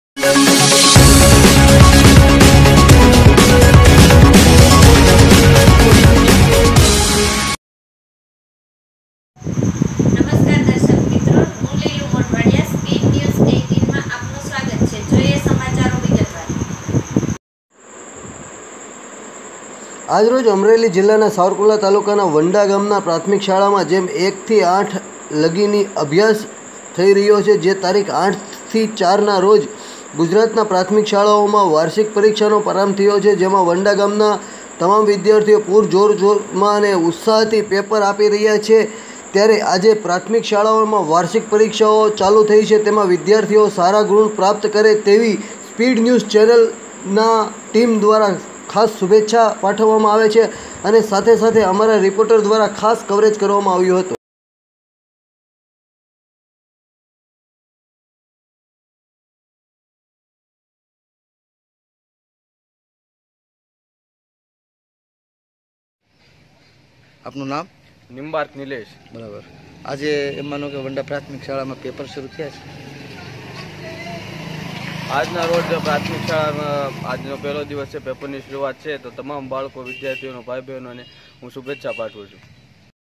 આજે પ્રાથમિક શાળાઓમાં વાર્ષિક પરીક્ષાઓ ચાલુ થઈ છે જેમાં વિદ્યાર્થીઓ સારા ગુણ પ્રાપ્ત કરે તેવી શુભેચ્
આજે પ્રાથમિક શાળાઓમાં વાર્ષિક પરીક્ષાઓ ચાલુ થઈ છે જેમાં વિદ્યાર્થીઓ સારા ગુણ પ્રાપ્ત કરે તેવી શુભેચ્છાઓ આશિષ સાવરકુંડલા તાલુકાના વંડા ગામની પ્રાથમિક શાળા જેમાં એક થી આઠ લગી નો અભ્યાસ થઈ રહ્યો છે આજે તારીખ 8 4 ના રોજ ગુજરાતભરમાં પ્રાથમિક શાળાઓમાં વાર્ષિક પરીક્ષાનો પ્રારંભ થયો છે જેમાં વંડા ગામના તમામ વિદ્યાર્થીઓ પૂરજોશમાં ઉત્સાહથી પેપર આપી રહ્યા છે રિપોર્ટ બાય અશોક રાવળદેવ વંડા